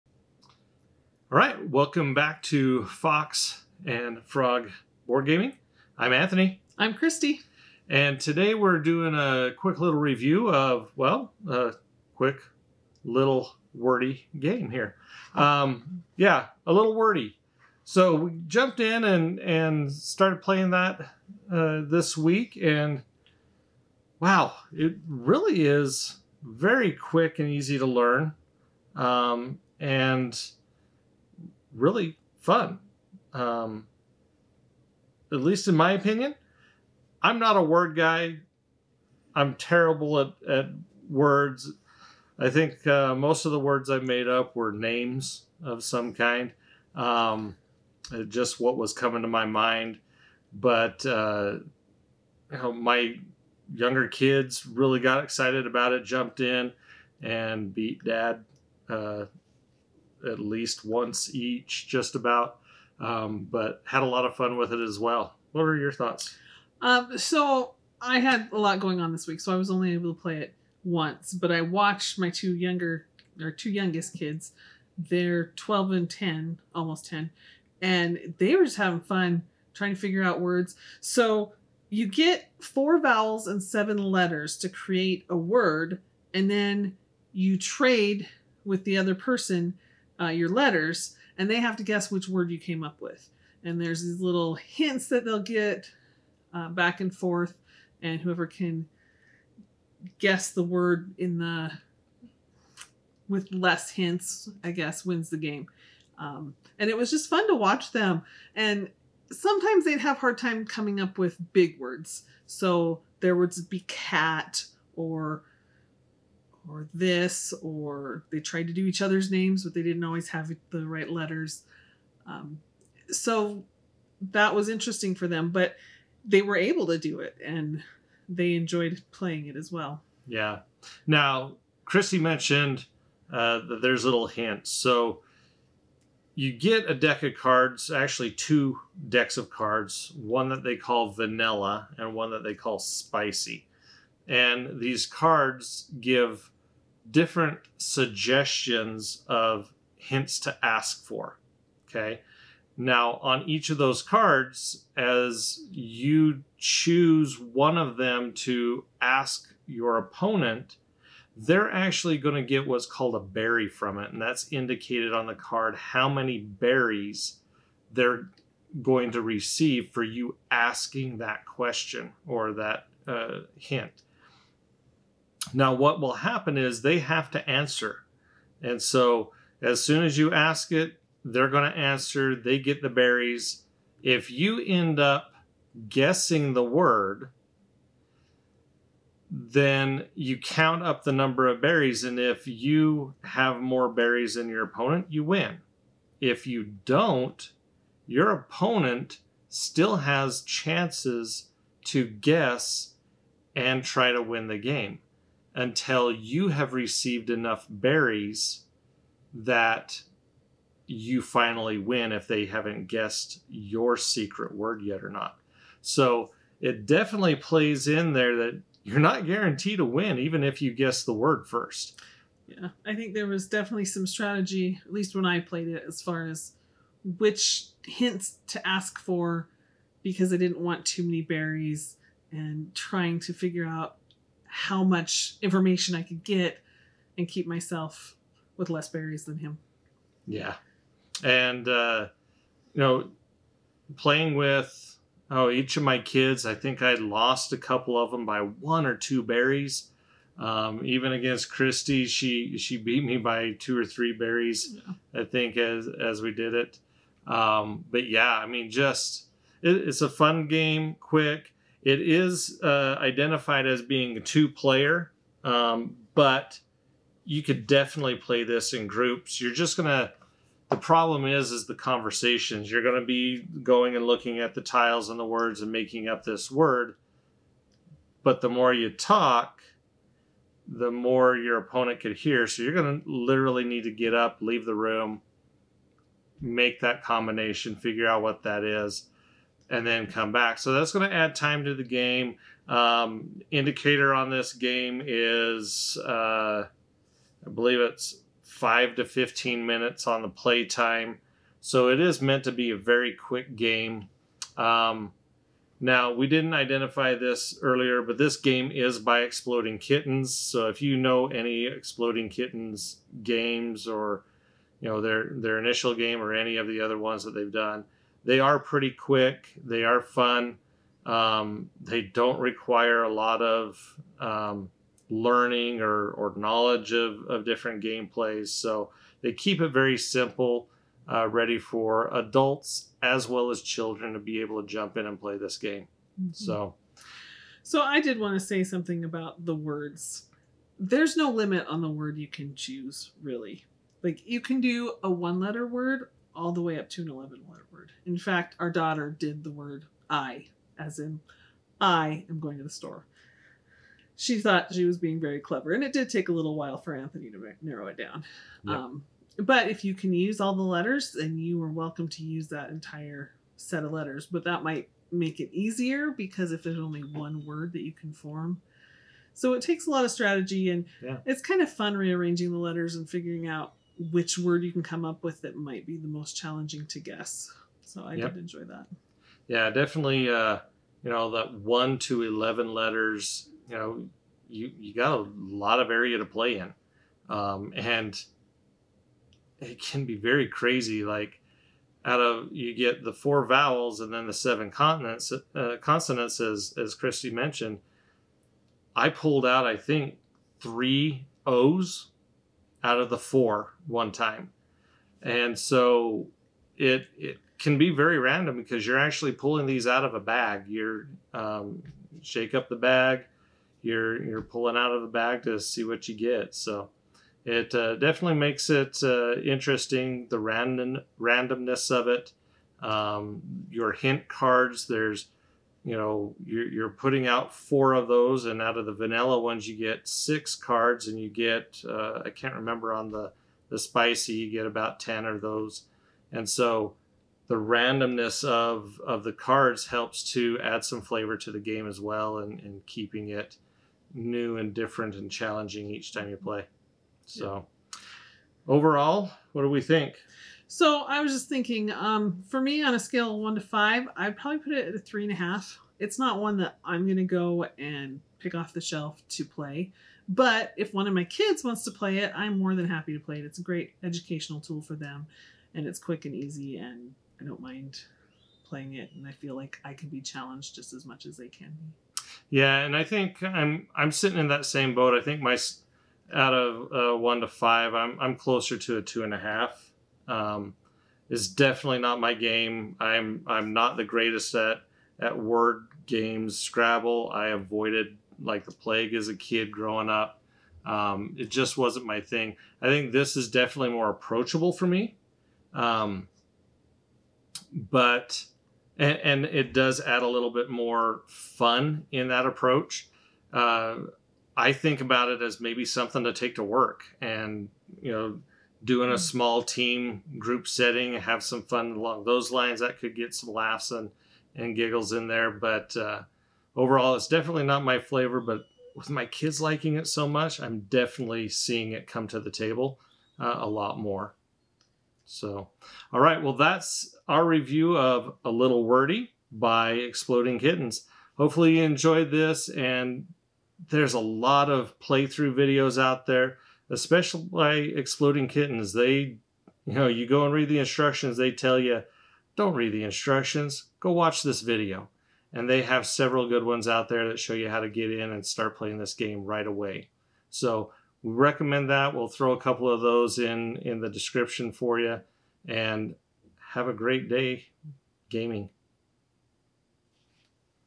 A Little Wordy Review
Fox and Frog reviews the 2 player game A Little Wordy game by Exploding Kittens.